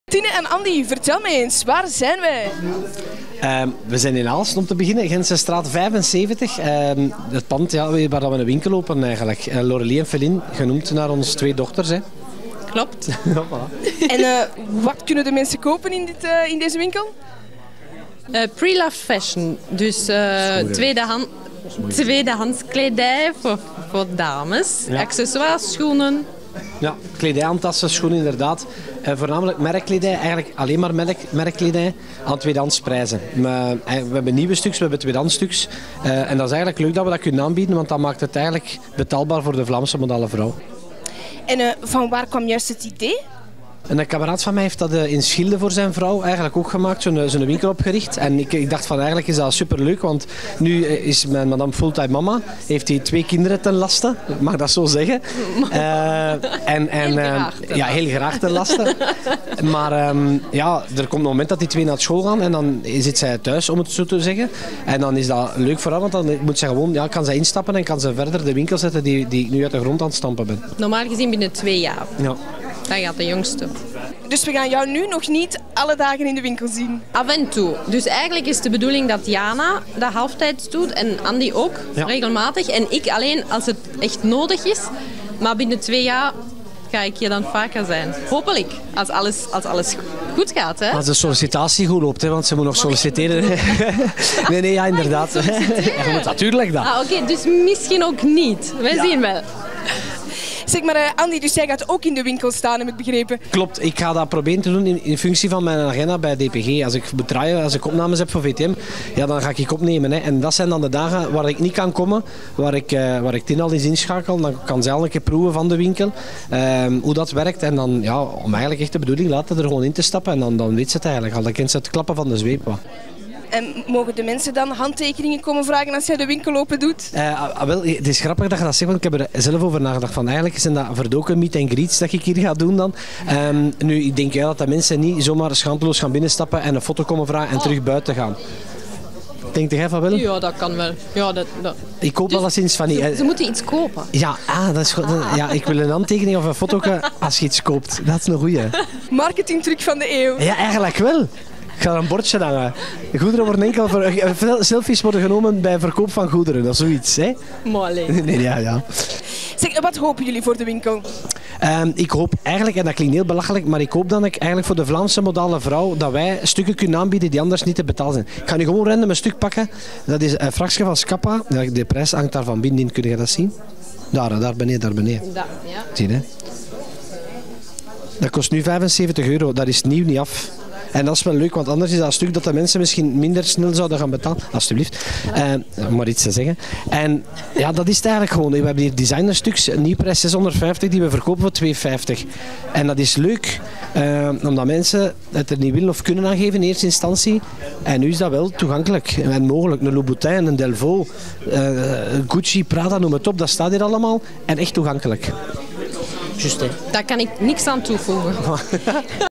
Andy Peelman opent winkel in Aalst: 'Wij maken Gucci en Louboutin toegankelijk'
Een winkel met tweedehandskledij is niet nieuw, maar dat je in de winkel modemerken als Gucci en Louboutin verkoopt aan meer toegankelijke bedragen, dat zou wel eens een succes kunnen worden. Andy Peelman is er al jaren in zijn hoofd mee bezig en nu brengt hij één en ander in de praktijk. Wij waren zaterdag op de plechtige opening in Aalst.